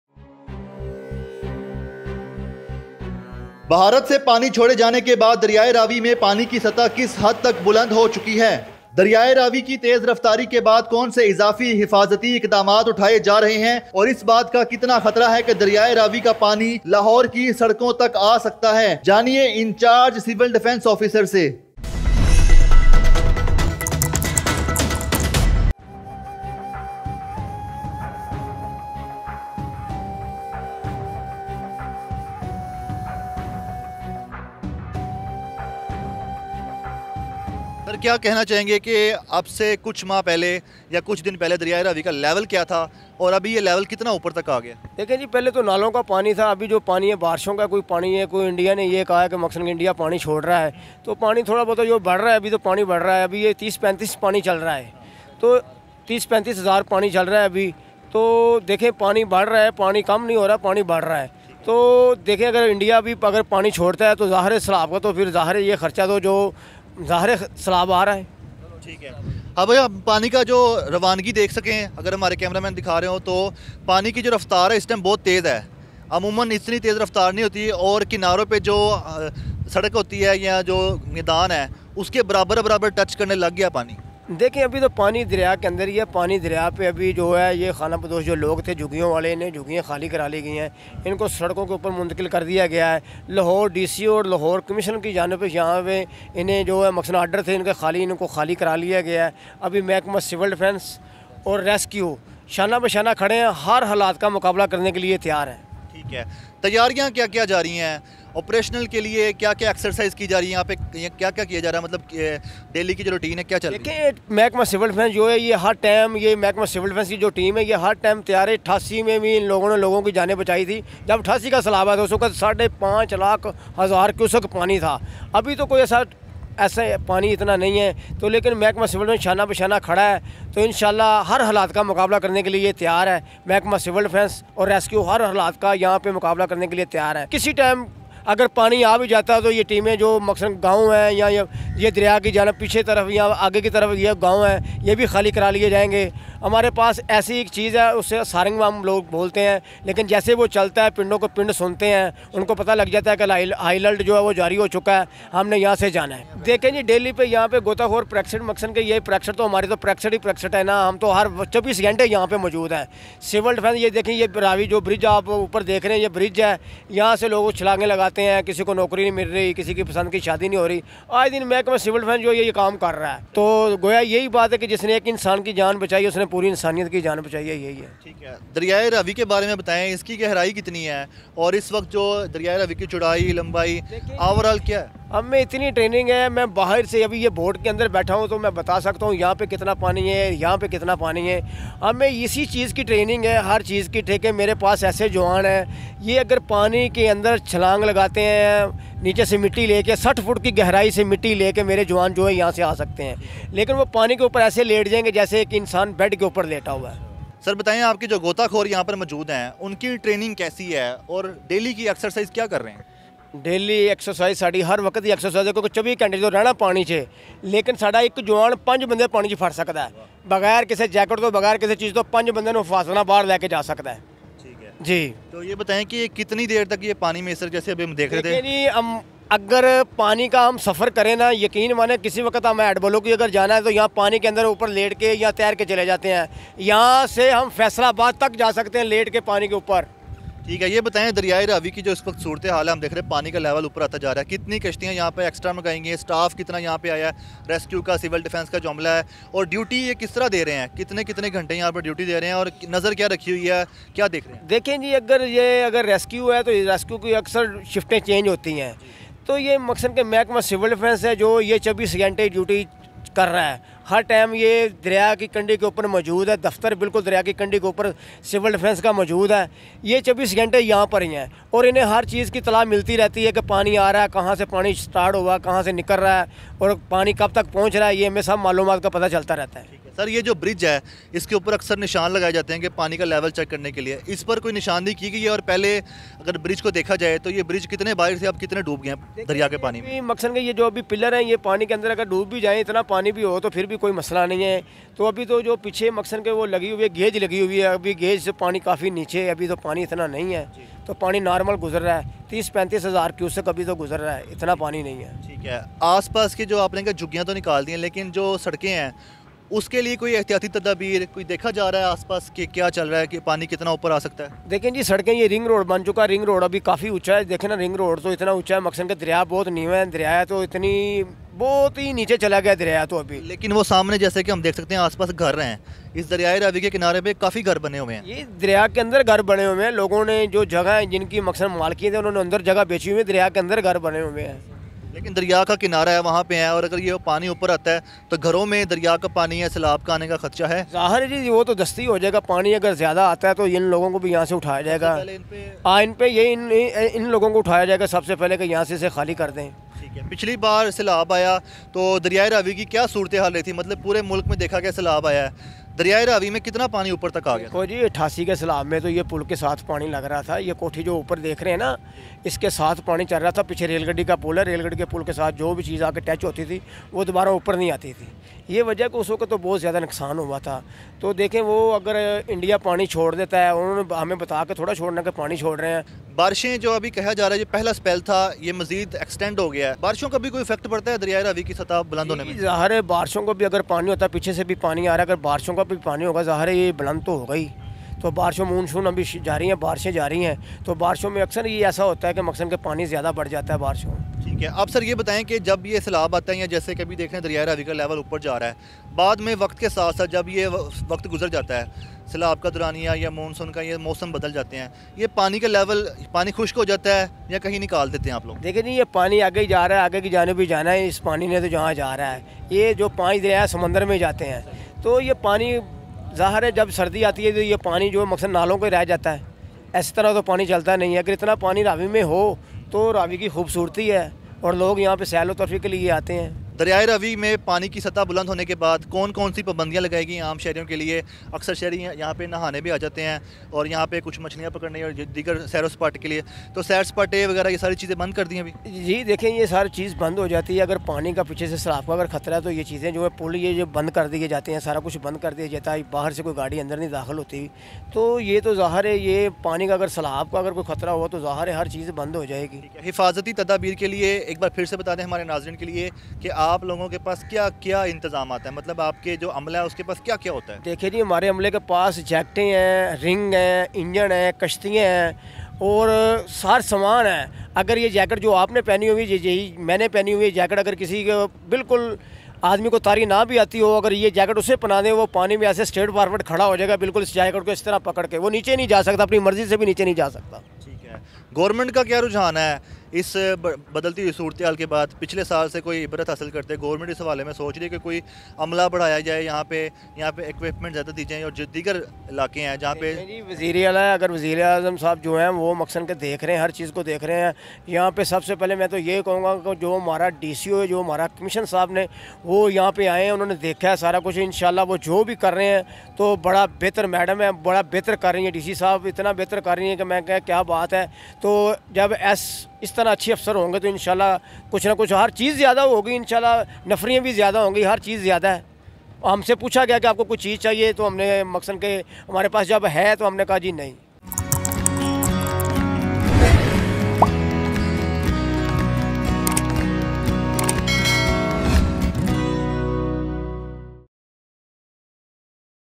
India Ne Ravi River Me Pani Chor Dia - Kia Ravi River Ka Paani Lahore Ki Roads Tak Aa Sakta Ha?
Heavy downpour in the Punjab region of Pakistan already created havoc in the country and on that India has warned to release water in River Ravi which has caused the fear of Flooding. UrduPoint anchor Usman butt has paid a Visit to river Ravi in the video and has given us the details of the precautionary measures to avoid any unwanted situation.
Anchor: Usman Butt